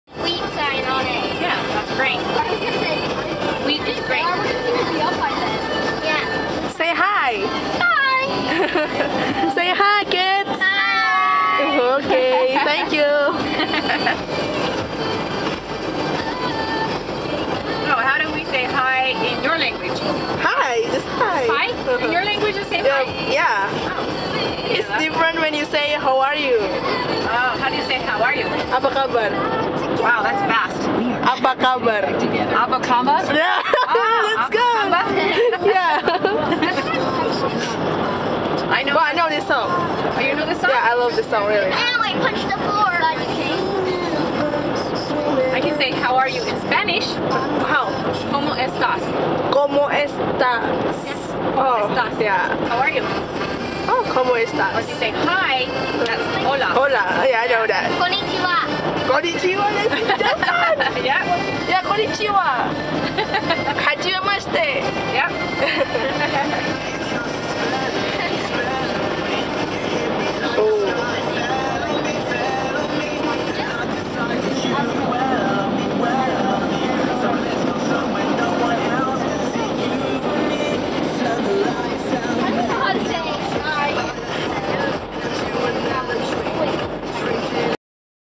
(Wahyuni Putri's - Layton, Utah) OTW Thanksgiving Point Part 2
It's on the way to Thanksgiving Point. Mom said "How are you?" in Indonesia :).
We went there with Shelby and Alexis, they are our neighbors :)